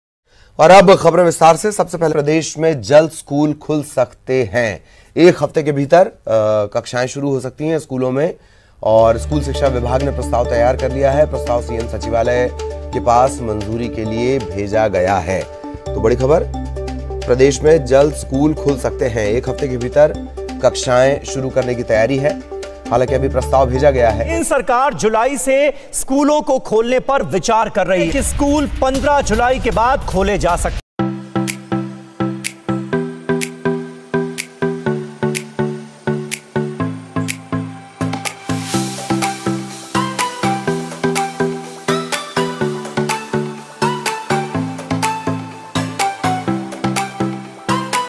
💥जल्द खुलेंगे स्कूल | School Reopen Date 2021 | School Kab Khulenge 2021 News Today
15 जुलाई से स्कूल खोलने की तैयारी | School Reopen Date 2021 | School Kab Khulenge 2021 Latest News